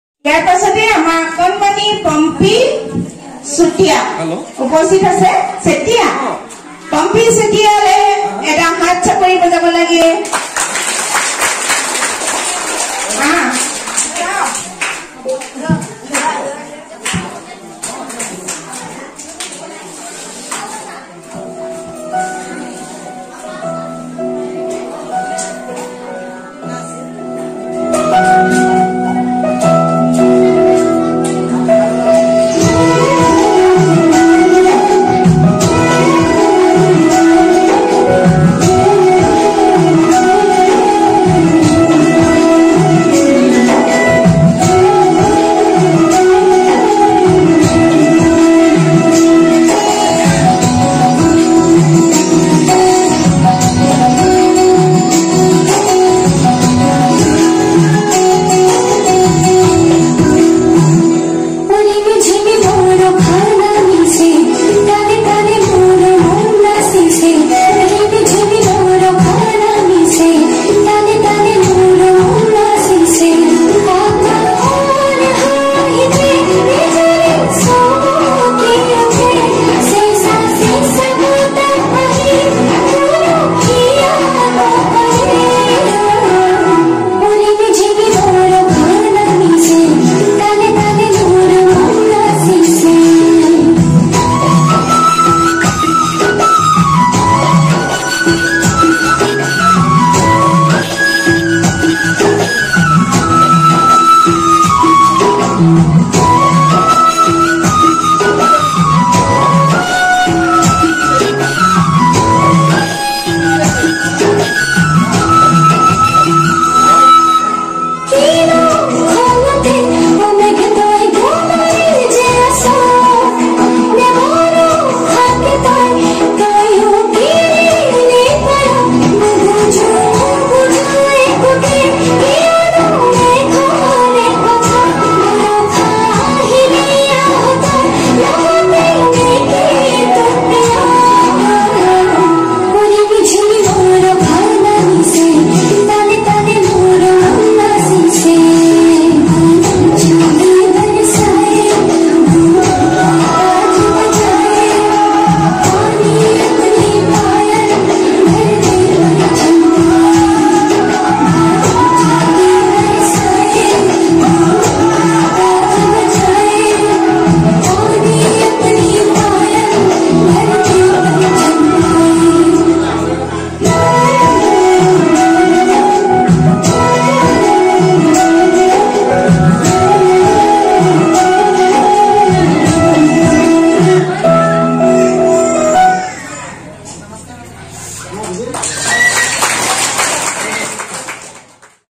দিব্যাঙ্গ দিৱস#December 3,2021#Ft.দিব্যাংগ সপ্তাহ উদযাপন#29/11/2021
আন্তৰ্জাতিক দিব্যাঙ্গ দিৱস#December 3,2021#अन्तर्राष्ट्रीय दिव्यांग दिवस Ft.দিব্যাংগ সপ্তাহ উদযাপন#29/11/2021
নিলখ শ্ৰীপাণী अन्तर्राष्ट्रीय दिव्यांग दिवस#International Day of Persons with Disabilities!
Ft.Sri.Abani Nath (DPO-IE)
SSA Dhemaji Block
District - Dhemaji.